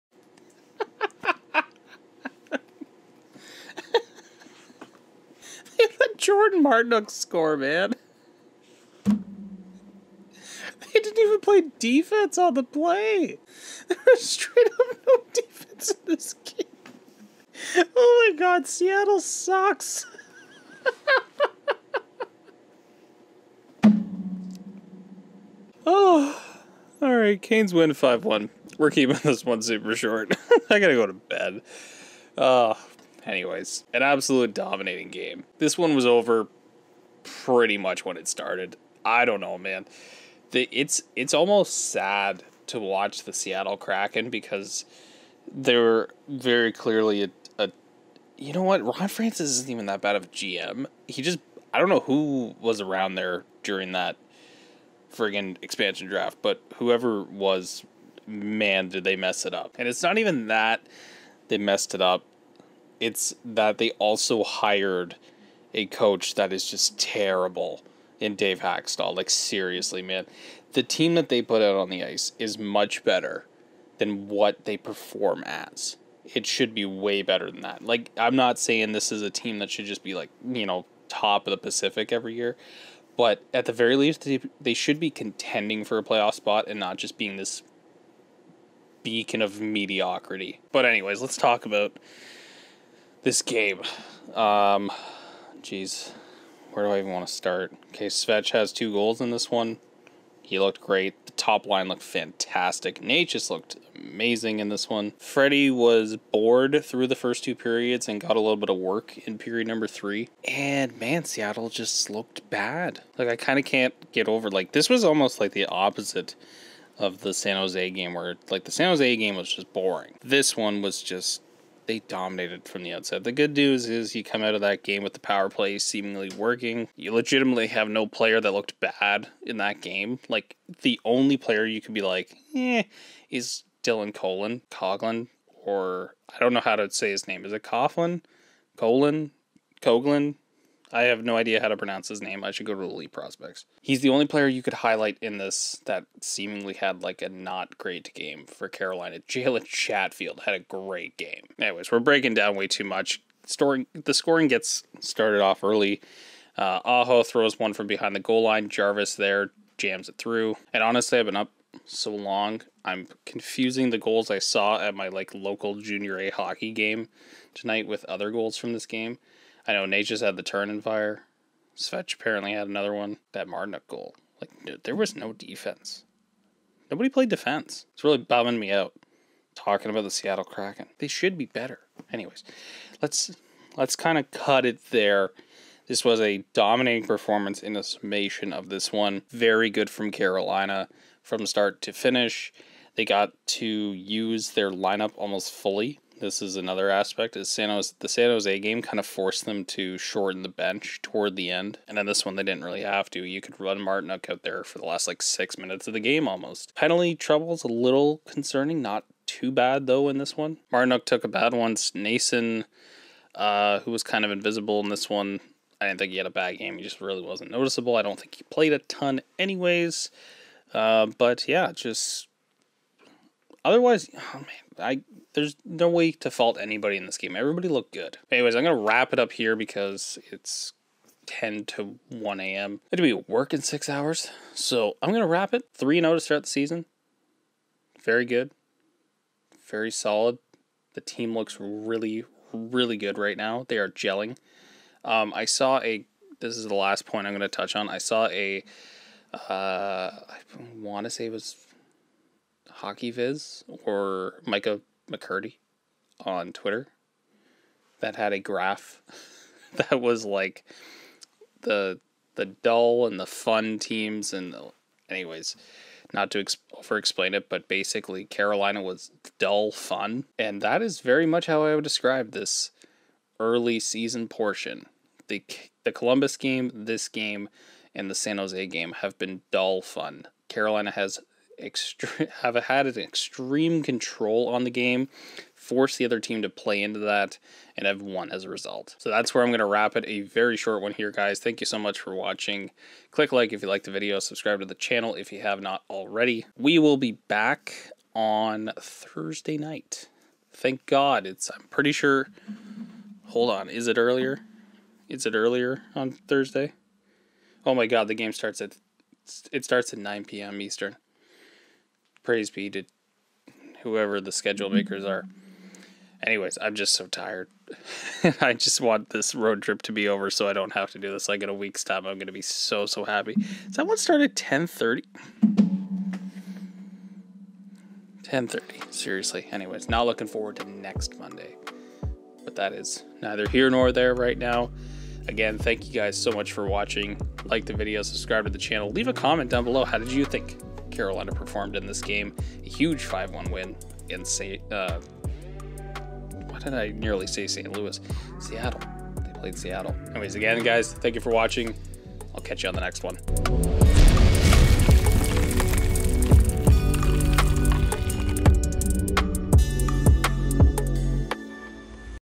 CGR 4 Game 3 | CAR 5 - 1 SEA | CGR.on.ATS
The Canes dominate the Kraken and improve to 3-0-0 on the season!